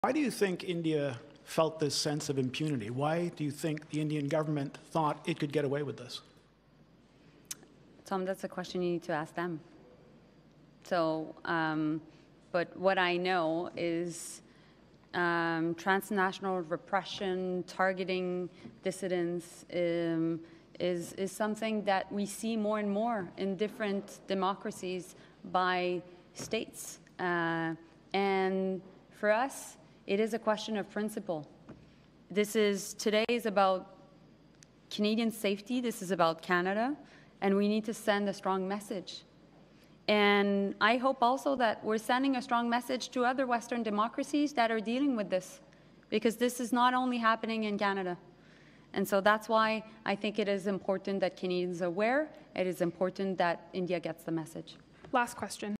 Mélanie Joly’s response to question about India’s Killing of Sikhs in Canada
Canadian Minister of Foreign Affairs Mélanie Joly’s response when asked by a reporter ‘why do you think India felt this sense of impunity?  why do you think the Indian government thought it could get away with this?’